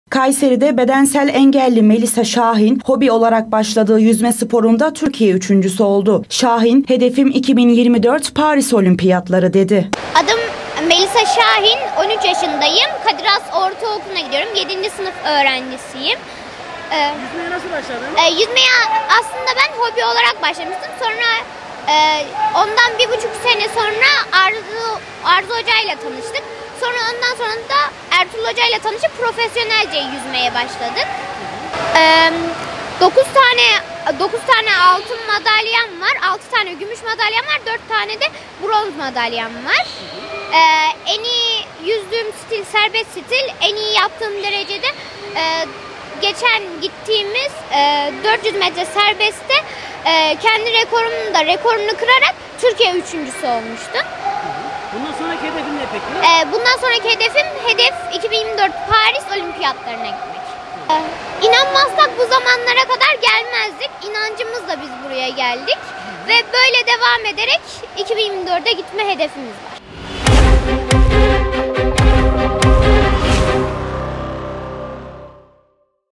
Engel tanımayan yüzücü Melisa Şahin’den Türkiye üçüncülüğü
KAYSERİ (1HA -ÖZEL HABER) Kayseri' de bedensel engelli Melisa Şahin (13), hobi olarak başladığı yüzme sporunda Türkiye üçüncüsü oldu. Şahin, "Hedefim 2024 Paris Olimpiyatları" dedi.

Kayseri'de yaşayan Melisa Şahin, sol bacağından ampute olarak doğdu. 7'nci sınıfa giden Şahin, hobi olarak başladığı yüzmede 19 madalya kazandı. Engelli bireyleri spora yönlendiren 'Mutlu Yüzler Projesi' Koordinatörü Arzu Bayram Eser ile tanışan Melisa Şahin 1HA'ya özel açıklamalarda bulundu. Melisa şahin konuşmasında, " Kadir Has Ortaokulu 7'inci sınıf öğrencisiyim. Yüzmeye hobi olarak başlamıştım. 1.5 sene sonra 'Mutlu Yüzler Projesi' Koordinatörü Arzu Bayram Eser ile tanıştık. Onun vesilesiyle de Ertuğrul hoca ile tanışıp lisanslı olarak yüzmeye başladık. Bu zamana kadar 9 altın, 6 gümüş ve 4 de bronz madalya kazandım. Hedefim Paris Olimpiyatlarına gitmek. İnanmazsak buralara gelmezdik, inancımızla buraya geldik. Böyle devam edip 2024 Olimpiyat Olimpiyatlarına gitmek istiyorum" diye konuştu.